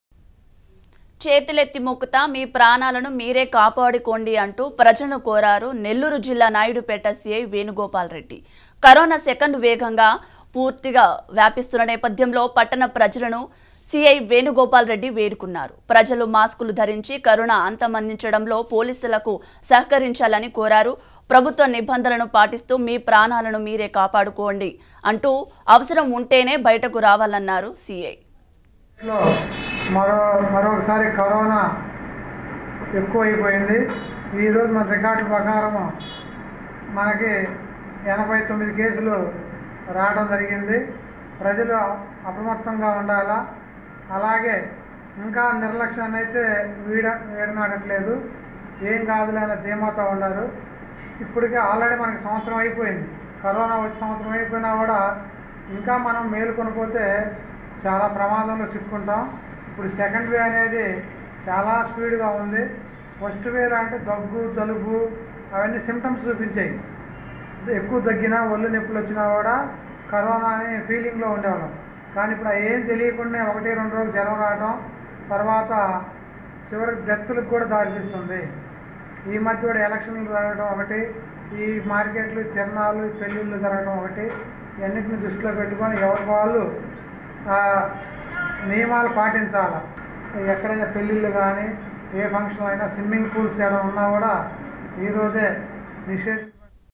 చేతులెత్తి మొక్కుతా..మీ ప్రాణాలు మీరే కాపాడుకోండి: CI Venugopal Reddy | Nellore | Raj News Telugu
చేతులెత్తి మొక్కుతా..మీ ప్రాణాలు మీరే కాపాడుకోండి: CI Venugopal Reddy on Covid-19 | Raj News Telugu